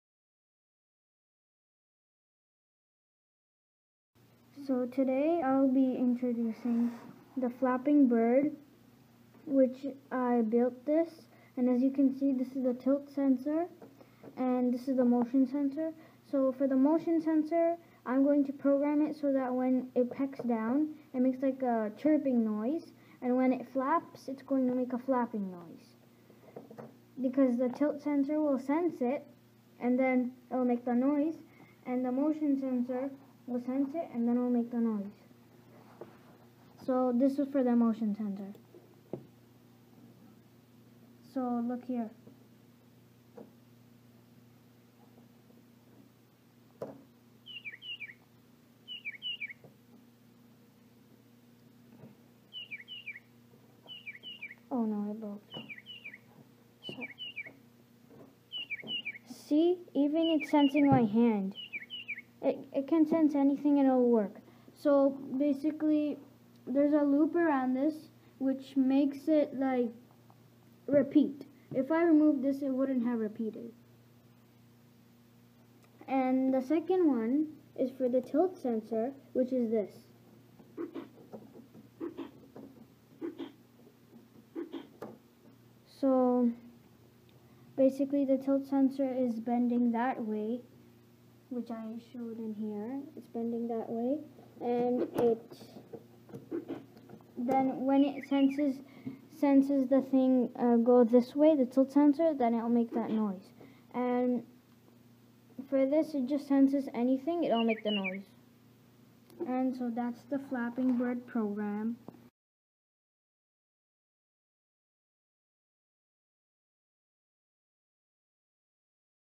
Lego WeDo Robotics: The Flapping Bird
Hey there! This is a video explaining the flapping bird project made using the Lego WeDo set.

And don't forget to check us out on Twitter as well! -